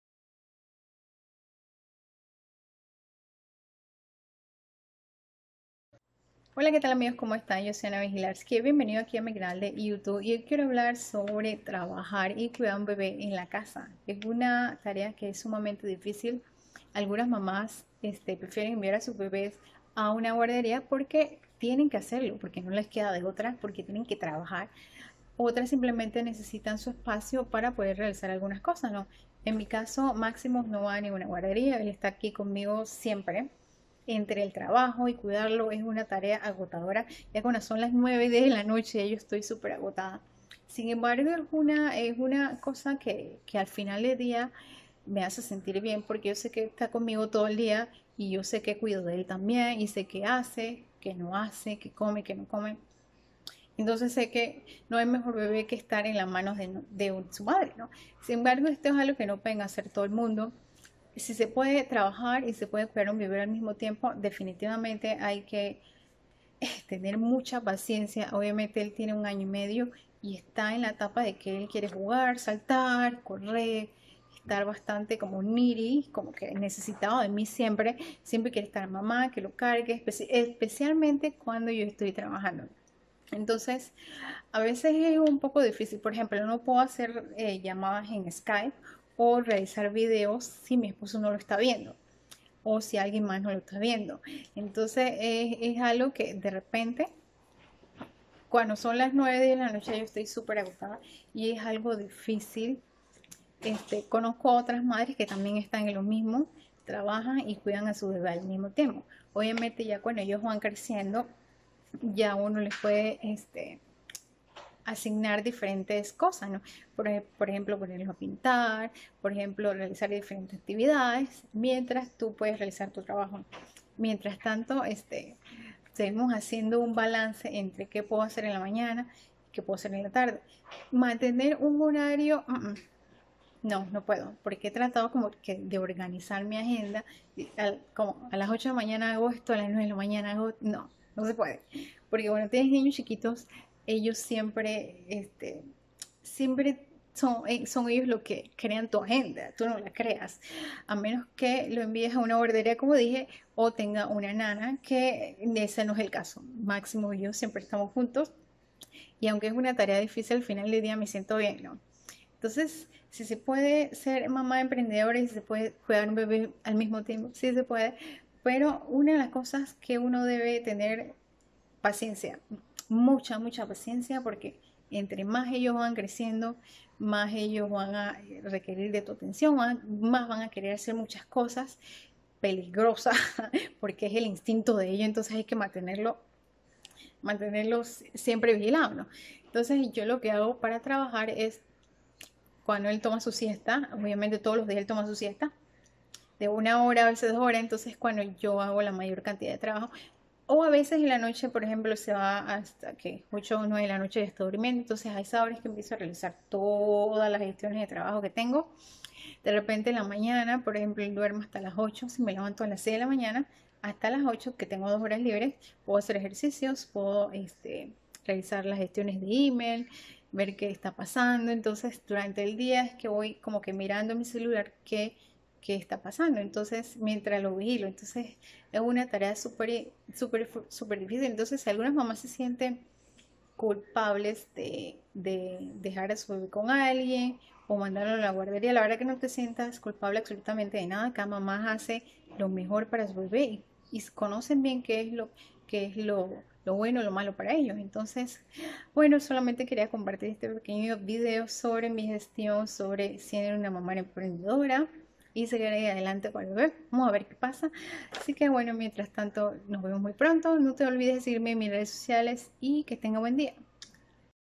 ¿Se puede trabajar desde casa y cuidar a tu bebe al mismo tiempo?
Maximus esta por cumplir dos años, etapa en la cual requiere de mucha atención. Desde que nació siempre he tratado de mantener un balance para seguir con mi etapa de mama y la de emprendedora.
Mantener un negocio y ser mama es realmente agotador y eso que tengo a mi esposo que me ayuda, sin embargo le tengo mucho respeto a las madres que hacen todo solas, ellas son el vivo ejemplo de que con esfuerzo todo se puede.

▼¿Deseas tener una mejor presencia online?